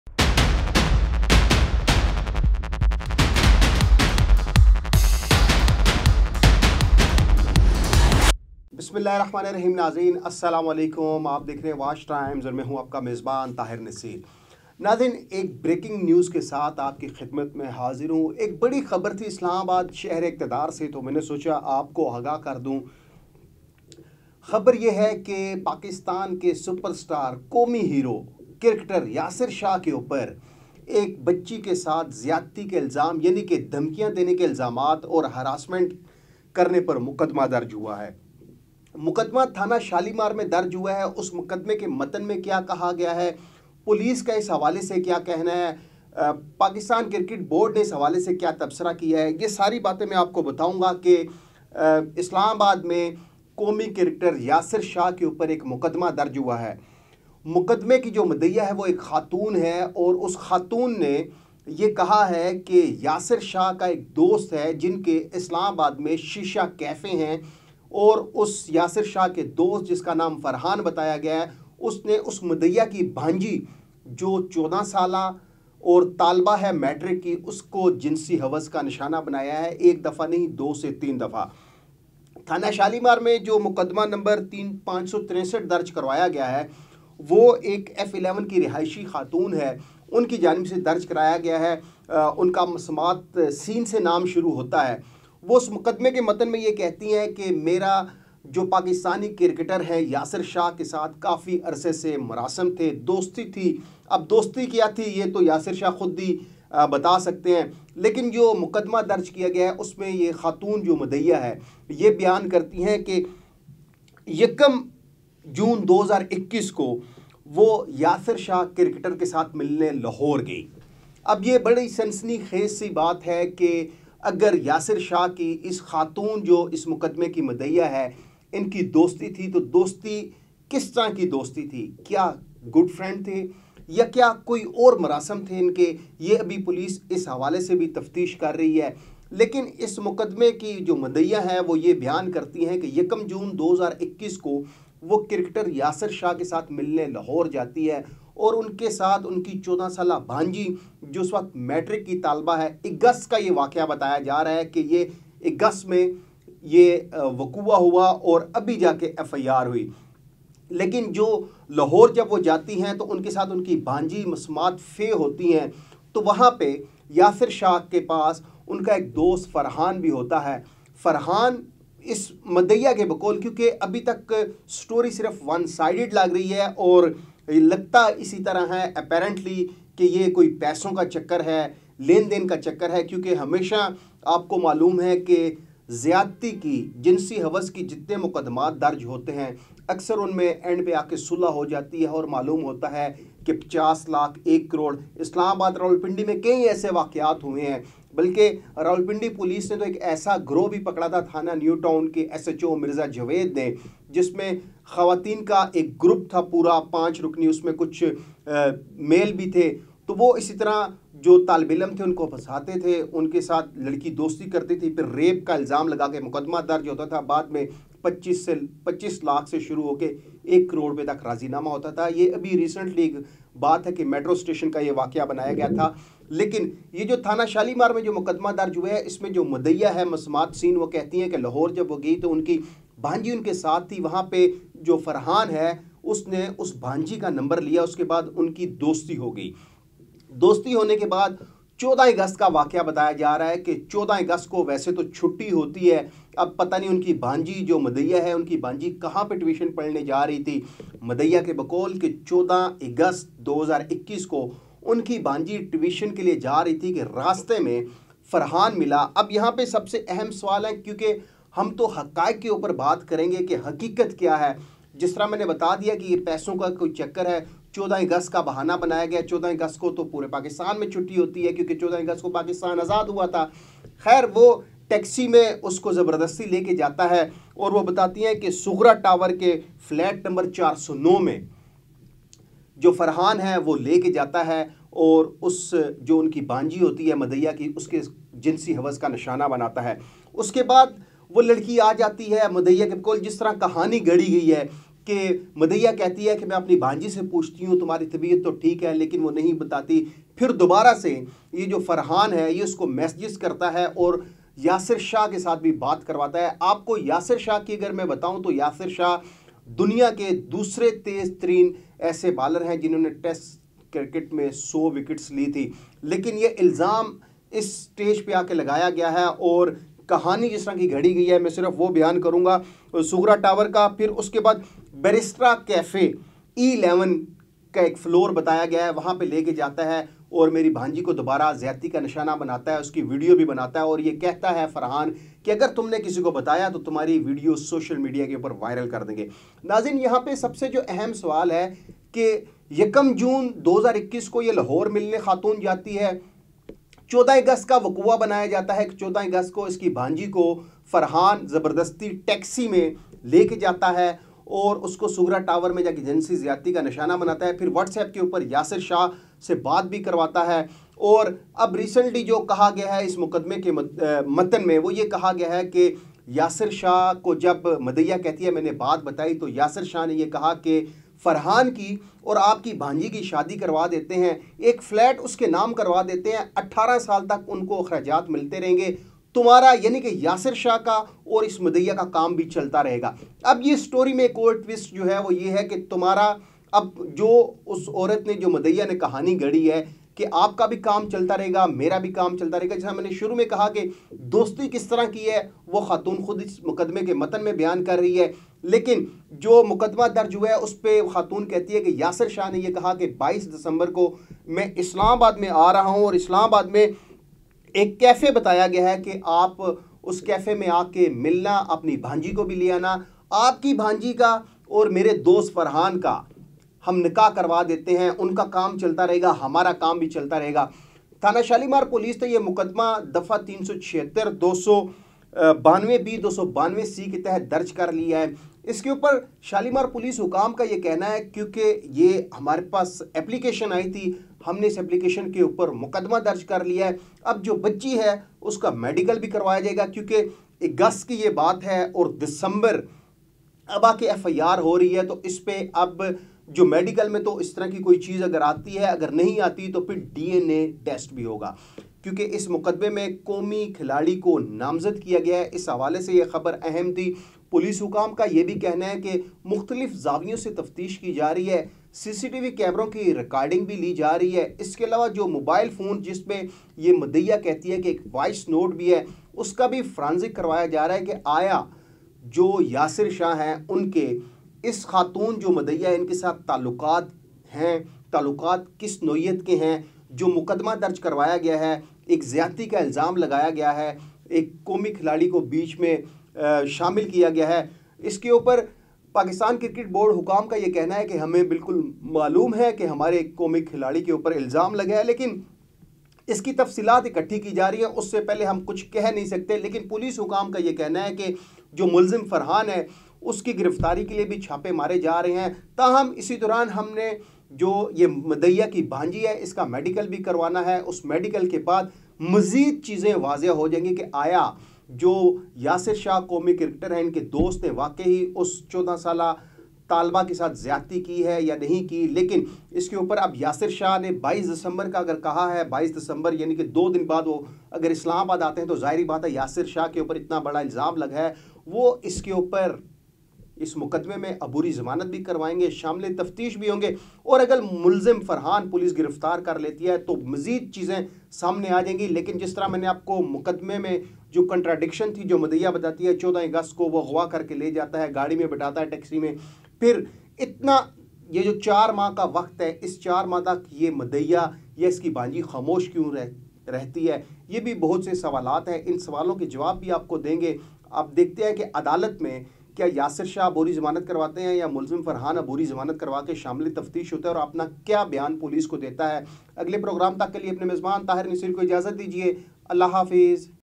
Yasir Shah Scandal in Islamabad | Ziyadti Allegation FIR Against Yasir Shah & his Friend
Yasir Shah Scandal in Islamabad Ziyadti Allegation FIR Against Yasir Shah & his Friend Pakistan cricket team leg spinner super star Yasir Shah in trouble